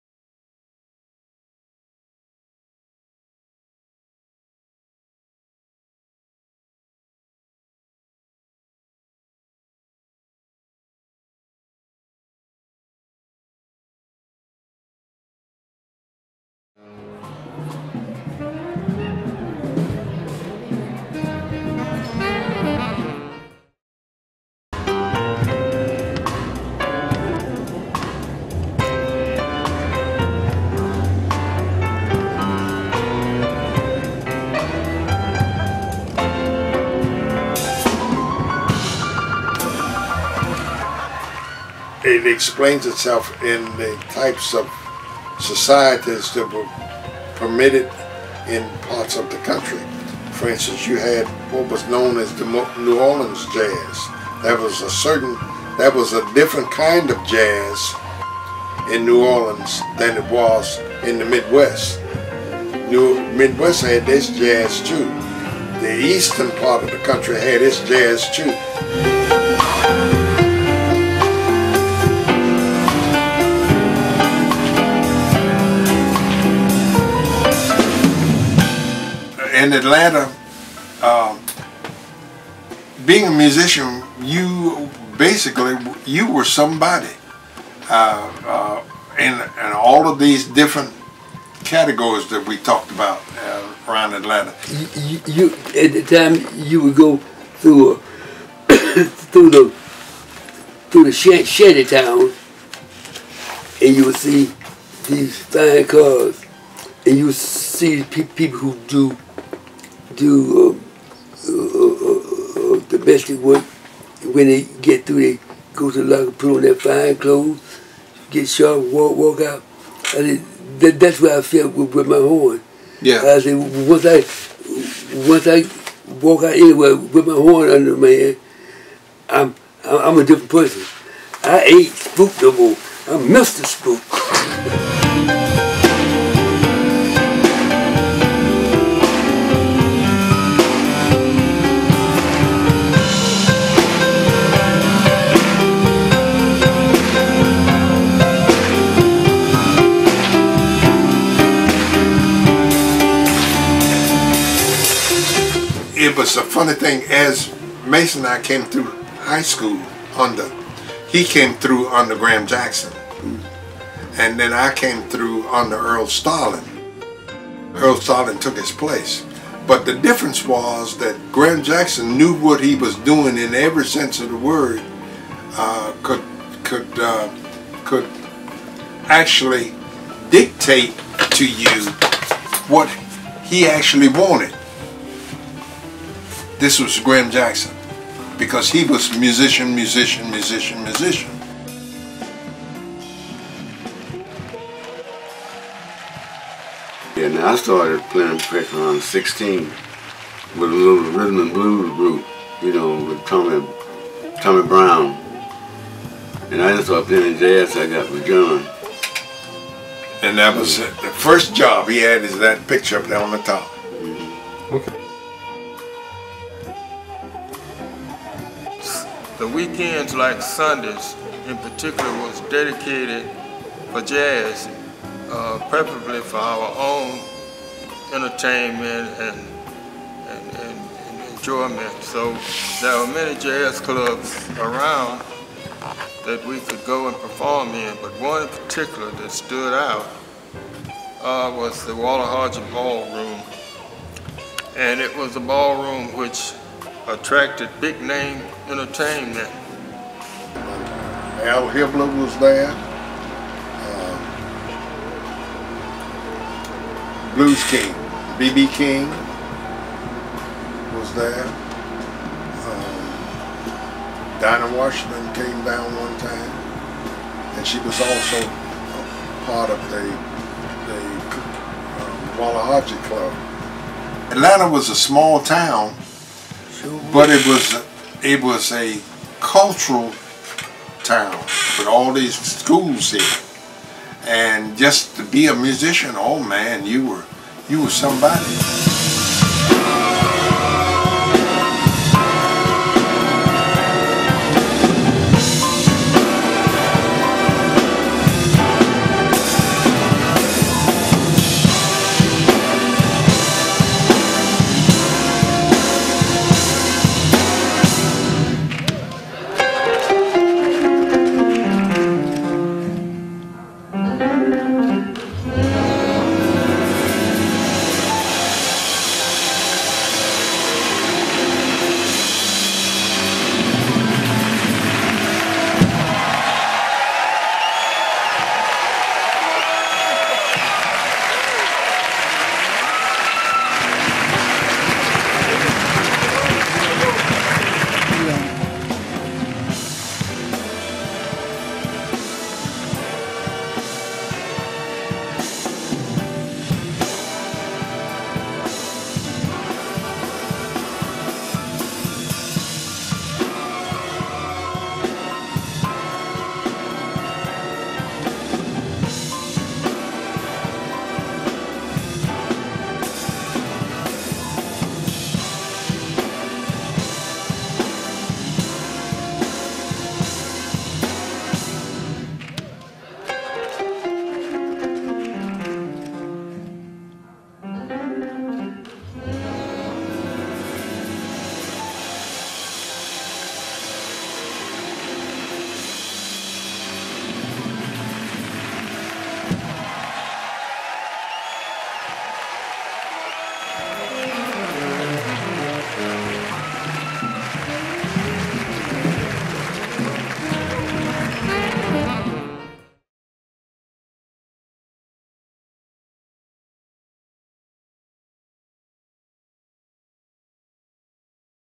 A Tale of Two Trumpets
A Tale of Two Trumpets is a small chapter of a larger story as it relates to the development of black music in the seat of the Confederacy. Evolving from Post Reconstruction to present day there has always been a multitiered social aspect of black life in Atlanta.

In this short bio pic, John Peek, Conductor, Director of the first African American Philharmonic Orchestra in the city of Atlanta takes us on a black cultural history tour. Mr. Peek is joined by friends and fellow musicians Mason "Spook" Johnson, Dub Hudson, and Billy Reid.

Harris Parson, Video Archivist